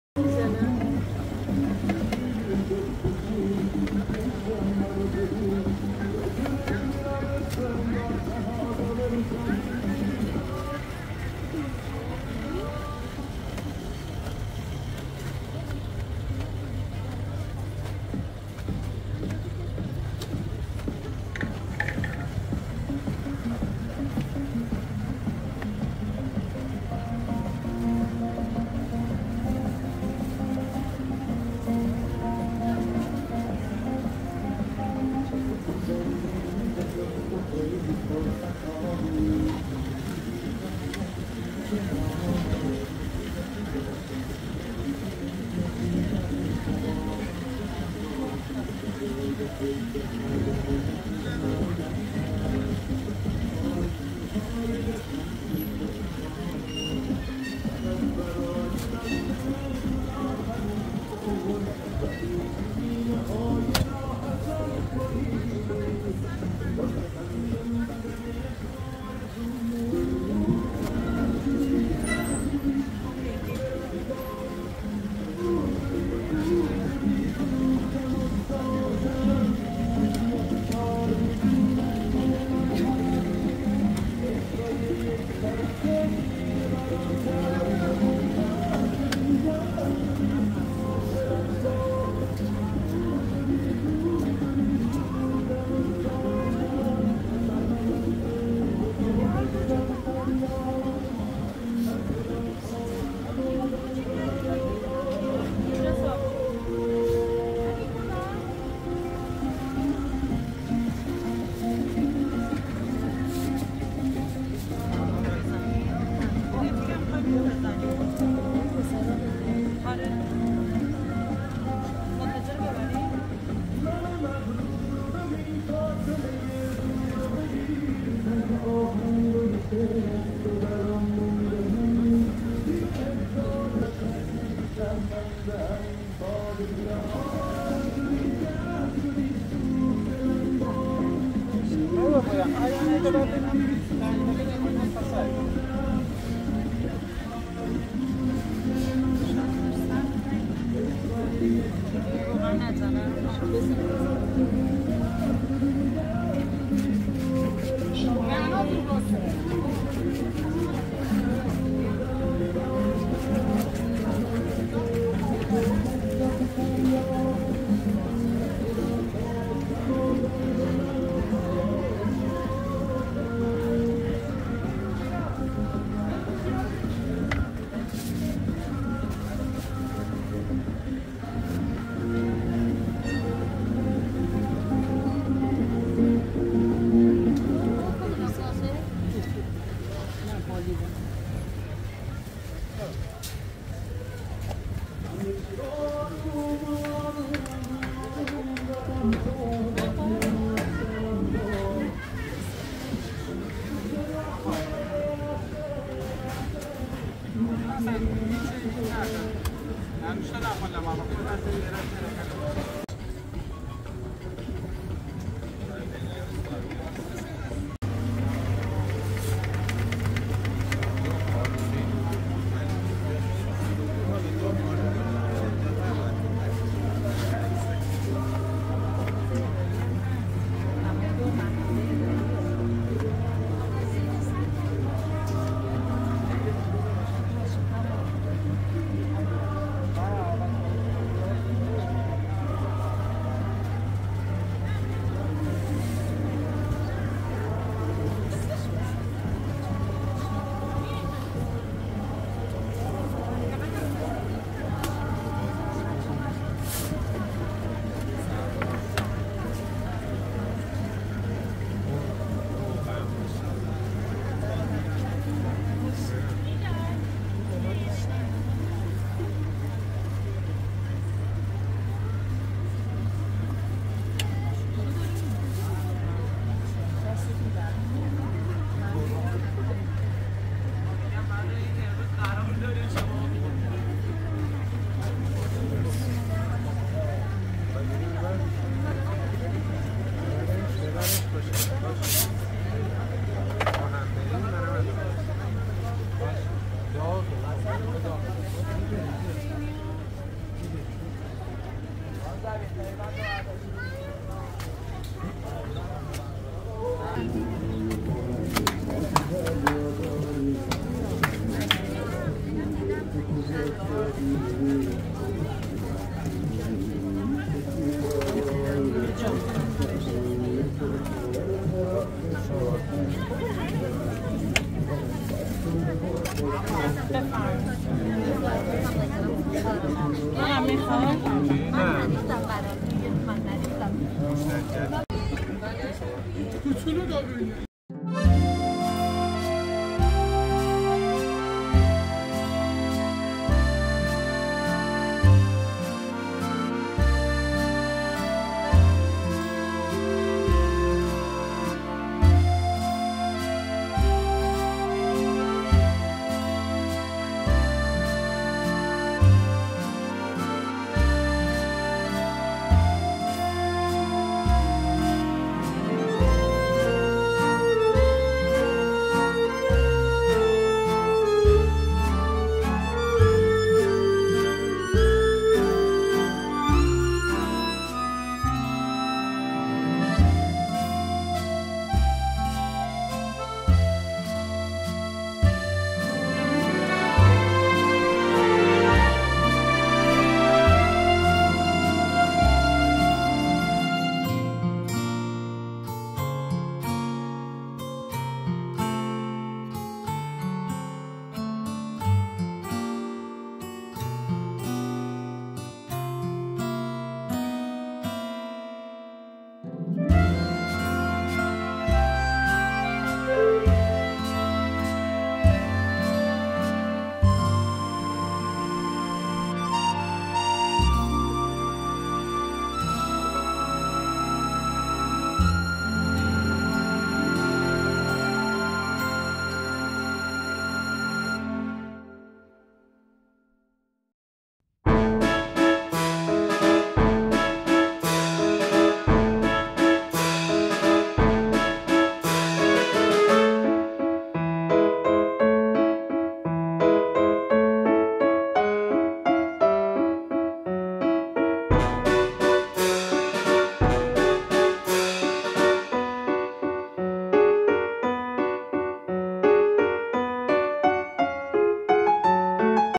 Baking Traditional Kurdestan Bread Kelana | Iran breads | Persian Breads
Kalane is one of the famous breads of Kurdistan and in this video, you can see how to make the dough and bake the bread

پختن نان کردی کلانه
کلانه یکی از نان های معروف کردستان هست و در این ویدیو درست کردن خمیر و پختن نان  را مشاهده میفرمایید

0:00 Baking Bread
5:38 Making Dough